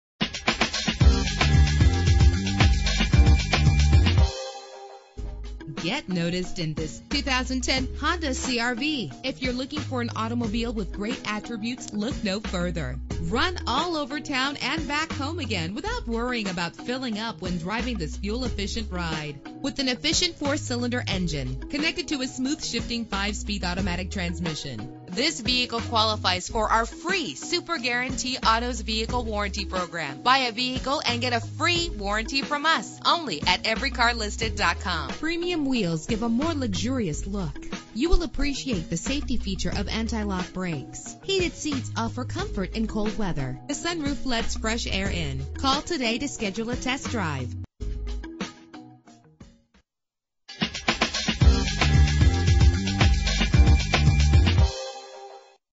2010 Honda CR-V - S Charleston WV
Year: 2010
Make: Honda
Model: CR-V
Trim: EX-L
Engine: 2.4 liter inline 4 cylinder 16 valve
Transmission: 5-Speed Automatic
Color: Blue
Mileage: 223
Address:
5102 MacCorkle Ave SW
S Char